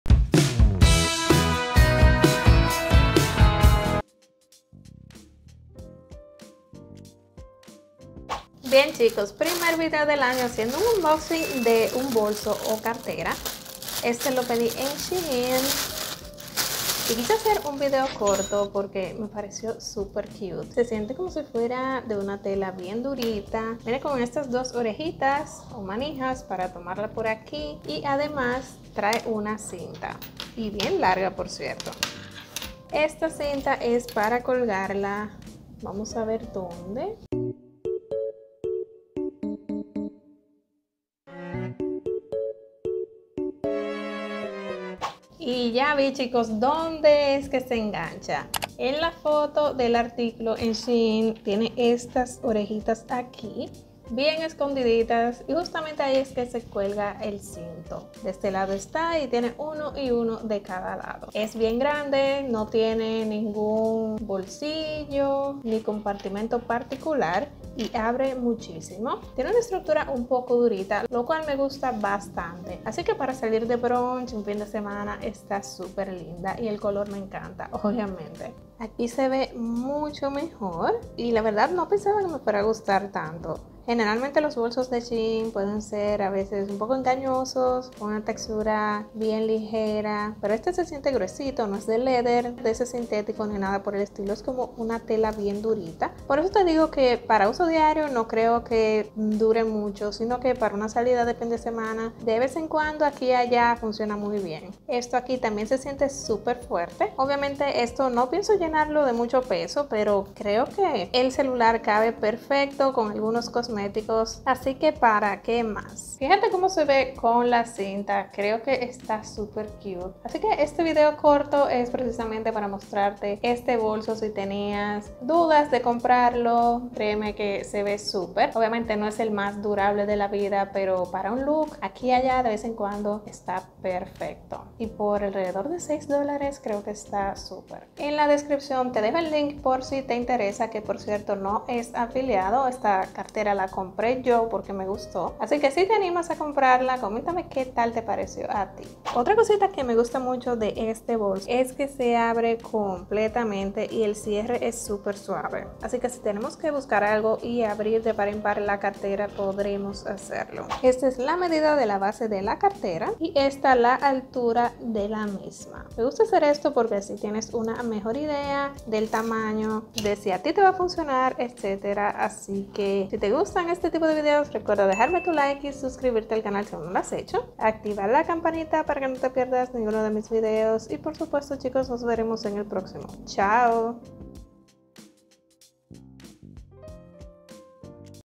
¡Bolso rosa de Shein!
¡Hola belleza! Estoy feliz de compartir contigo el unboxing de este bolso rosa que encontré en Shein.